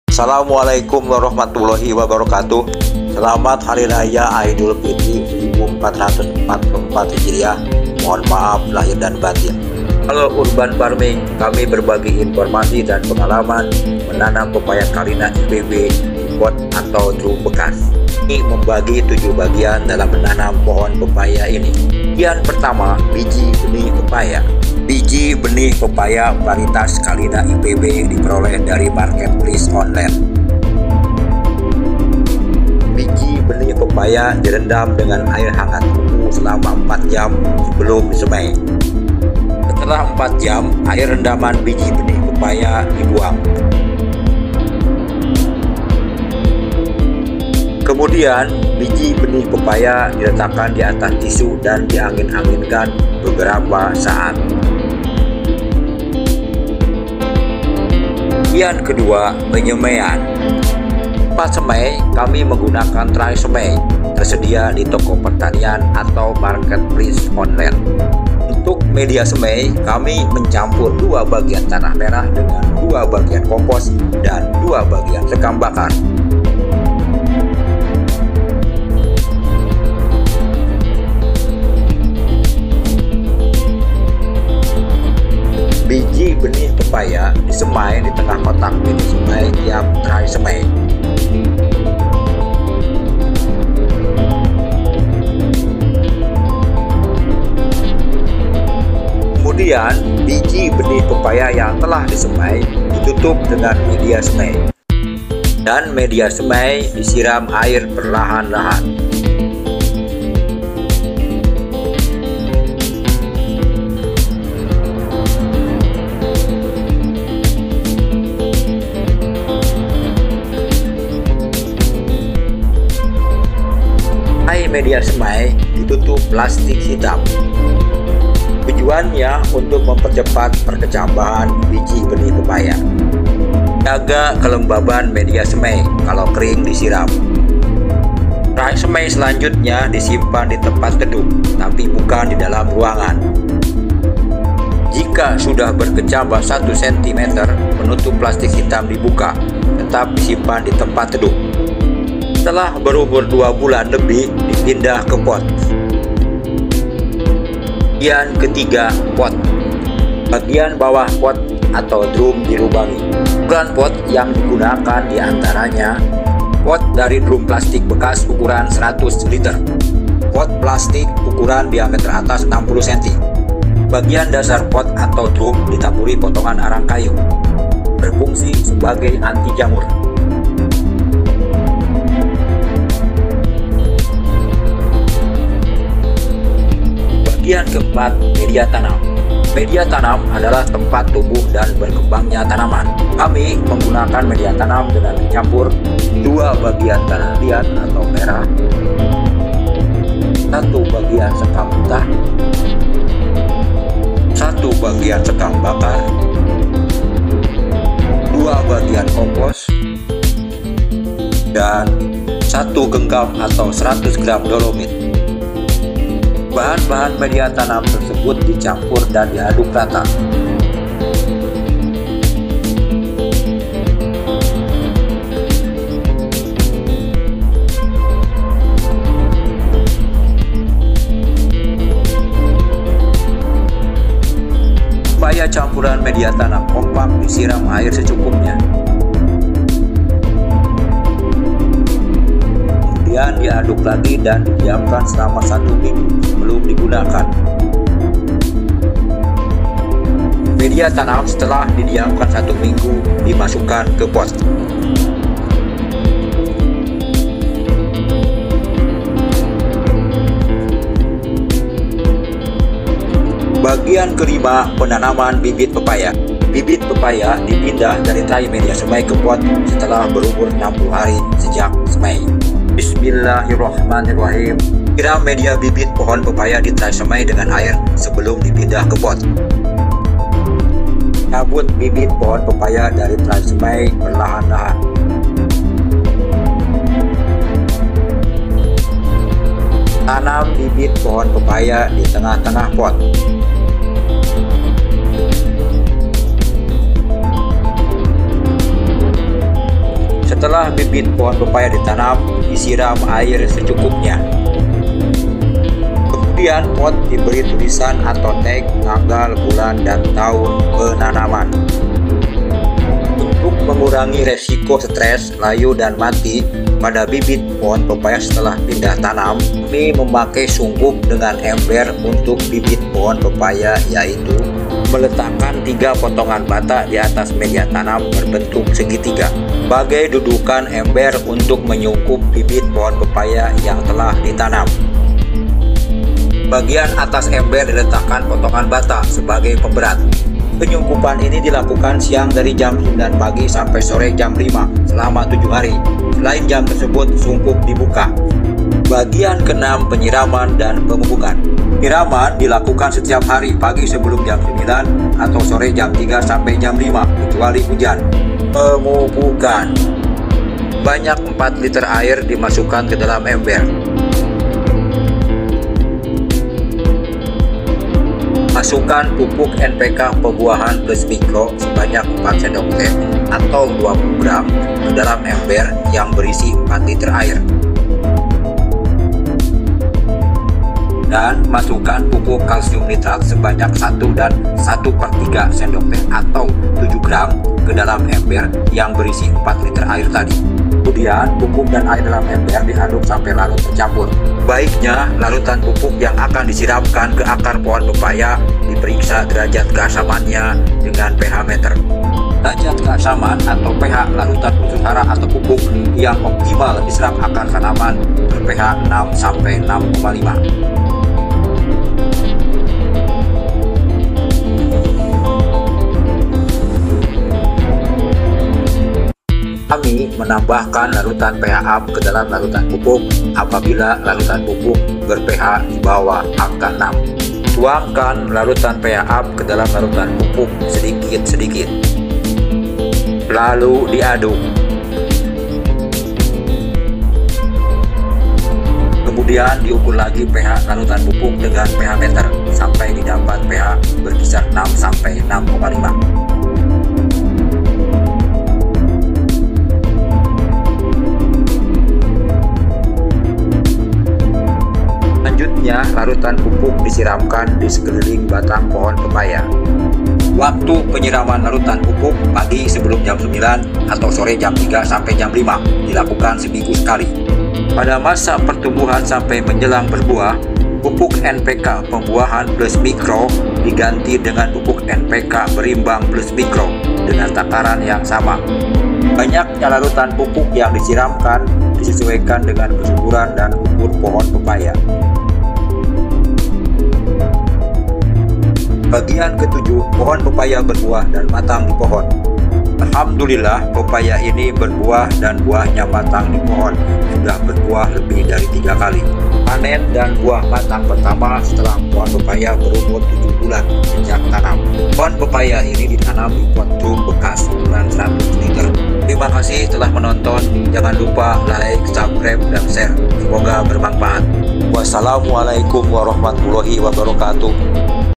Tanam Pepaya di Pot dari Biji agar Cepat Berbuah bagi Pemula
Demi kesuksesan menanam pepaya di pot, Anda perlu melakukan proses menanam pada pohon pepaya Anda seiring waktu. Pohon pepaya ini varietas callina IPB. Dalam video ini, Dadang Urban Farming berbagi pengalaman dan tips tentang Cara Menanam Pepaya di Pot dari Biji yang meliputi cara penyemaian biji pepaya, jenis dan ukuran pot yang digunakan, membuat media tanam yang digunakan, penanaman bibit pohon pepaya, penyiraman, pemupukan, buah pepaya matang di pohon dan panen, silahkan simak sampai habis videonya.

- Bibit dalam bentuk biji pepaya callina IPB
  diperoleh dari marketplace online.
- Media tanam untuk menanam pepaya :
  Tanah merah, sekam mentah / bakar dan
  kompos ( daun & ranting yg sdh jadi kompos) di
  peroleh dari toko pertanian atau marketplace online
- Dolomit adalah sejenis kapur pertanian
  diperoleh di toko pertanian atau marketplace online,
  kegunaannya untuk  meningkatkan pH tanah
  serta menetralkan kadar keasamannya.
- pH meter alat untuk mengetahui asam atau
  basa media tanam / larutan, banyak dijual di
  toko pertanian dan marketplace online.
- Pot dari drum plastik ukuran 200 liter yang
  dipotong menjadi 2, diperoleh dari lapak barang
  bekas, pot plastik biasa ukuran diameter
  atas 60 cm, tersedia di Toko Pertanian atau
  Marketplace online, bagian bawah pot, drum
  bekas dilubangi.
 - Penyiraman dilakukan 1 kali sehari , pagi
   sebelum jam 9 dan sore jam 15 ~ 17
 - Pemberian pupuk masa pertumbuhan sampai
   menjelang pembuahan : Pupuk NPK
   berimbang plus mikro ( 16-16-16 atau
   sejenisnya ) sebanyak 1 sendok teh ( 5 gram )+
   Pupuk kalsium nitrat ( CaO 26 % ,N 15 %, B 0,3
   % ) sebanyak 2 gram.
- Pemberian pupuk masa pembuahan :
  Pupuk NPK pembuahan plus mikro ( 15-09-20
  atau 15-10-30 ) sebanyak 1 sendok teh (5 gram
  ) + Kalsium nitrat ( CaO 26 %, N 15 %, B 0,3 % )
  sebanyak 2 gram.
 * Jenis pupuk granule/ butir atau bubuk
    dicampur dan dilarutkan dengan 1 liter air
    kemudian dikocorkan didekat batang
    pohon pepaya diberikan seminggu sekali, dosis
    disesuaikan dengan kesuburan & umur
    pohon pepaya.
 Semoga bermanfaat.
Jika ada pertanyaan , silahkan dikolom komentar ( Insya Alloh dijawab )

TONTON JUGA :
CARA STEK POHON BUAH SURGA TIN, FIG DAN BUAH ARA | BUAH YANG TERTULIS DALAM AL – QUR’AN :
COCOK DI TANAM DI POT TIN, FIG DAN ARA | BUAH POHON SURGA YANG BISA TUMBUH DI DUNIA :